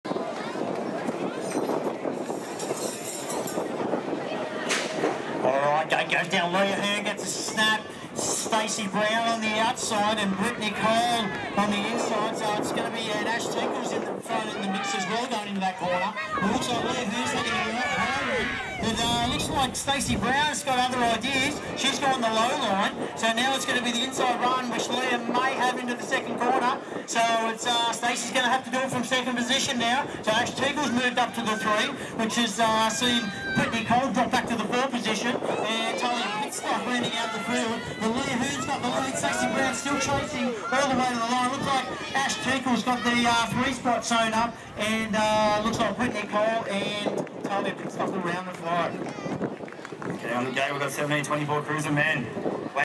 17-24 Women Cruiser Final - 2019 Lake Macquarie BMX Open
Round 1, BMXNSW State Series
3rd February 2019